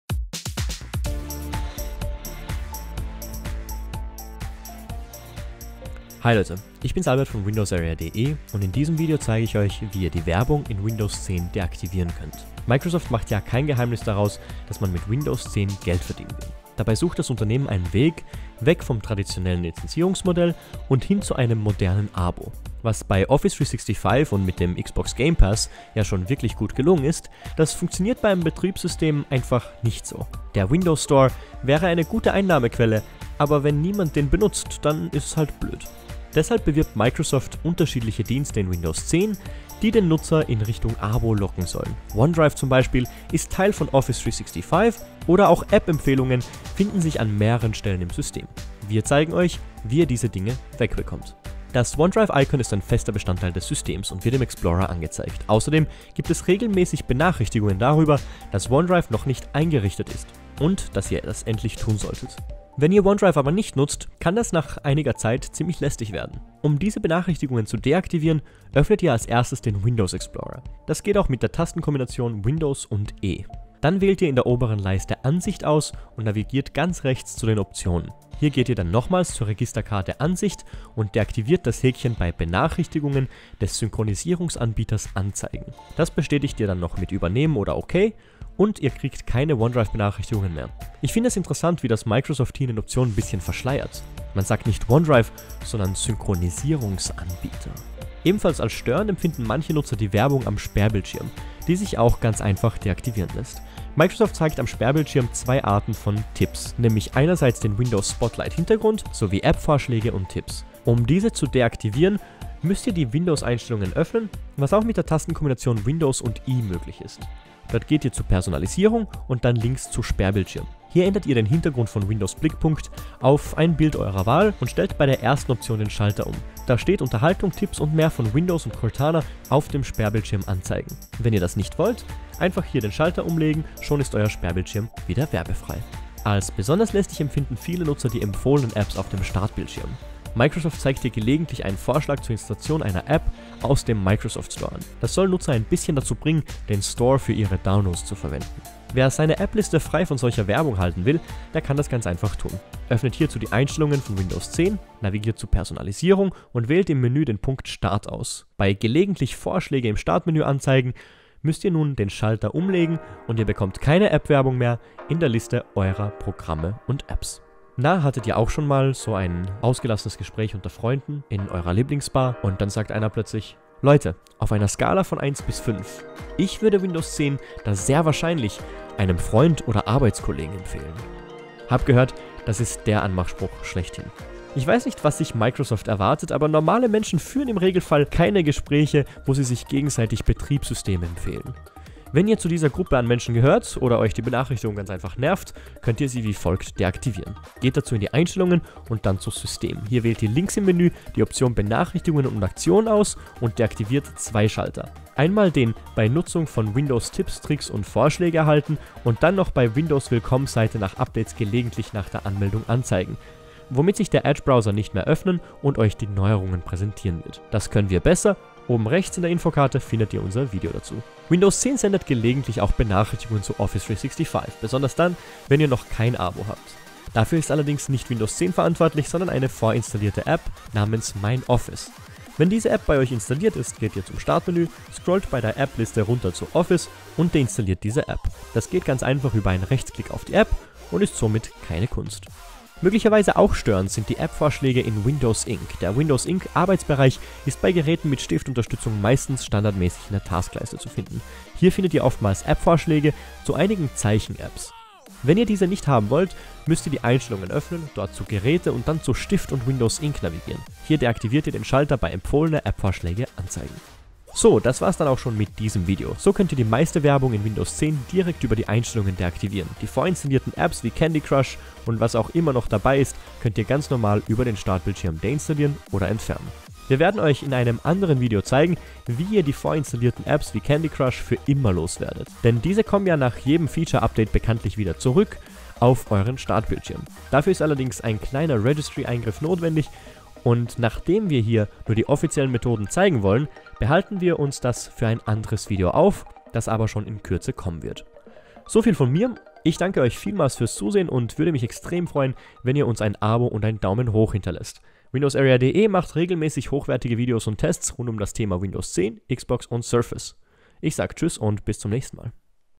Anleitung Sämtliche Werbung in Windows 10 entfernen
In diesem Video zeigen wir euch Schritt für Schritt, wie ihr sämtliche in Windows 10 integrierte Werbung deaktivieren könnt.